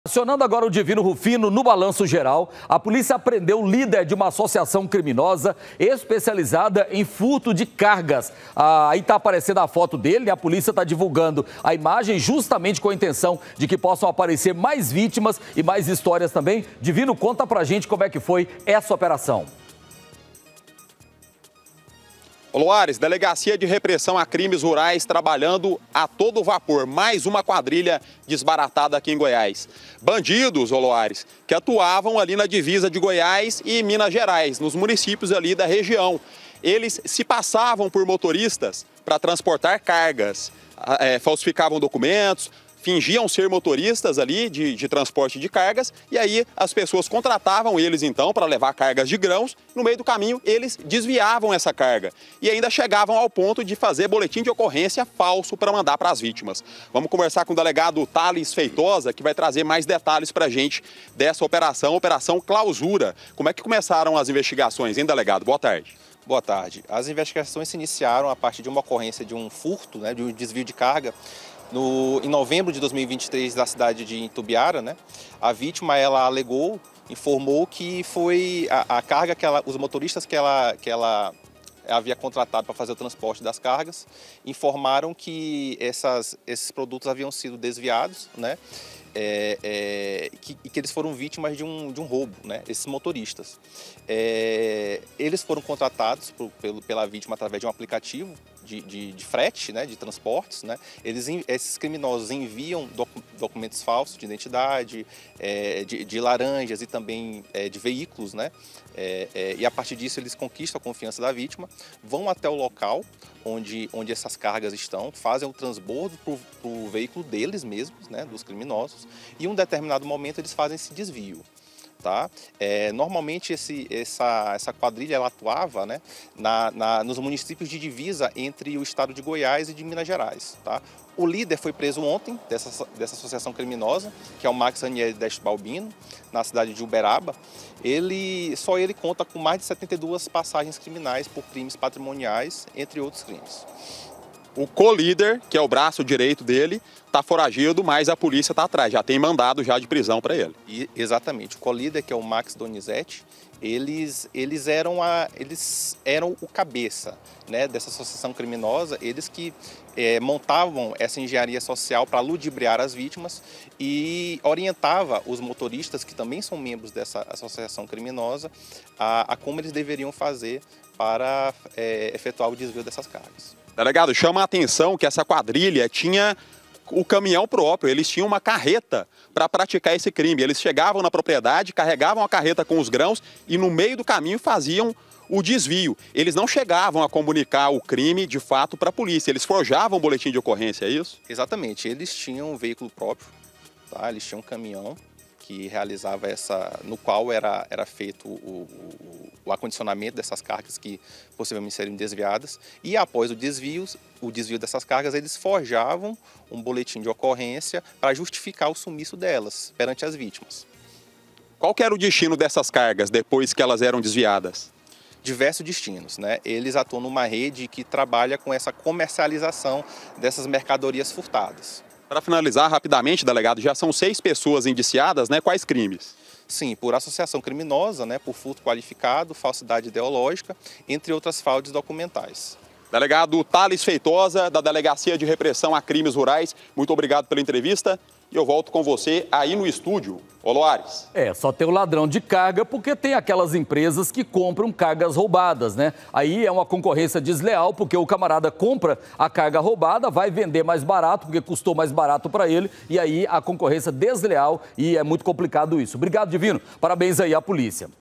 FINGIA SER MOTORISTA: POLÍCIA PRENDE LÍDER DE ASSOCIAÇÃO CRIMINOSA ESPECIALIZADA EM FURTOS DE CARGAS
FINGIAM SER MOTORISTAS: POLÍCIA PRENDE LÍDER DE ASSOCIAÇÃO CRIMINOSA ESPECIALIZADA EM FURTOS DE CARGAS.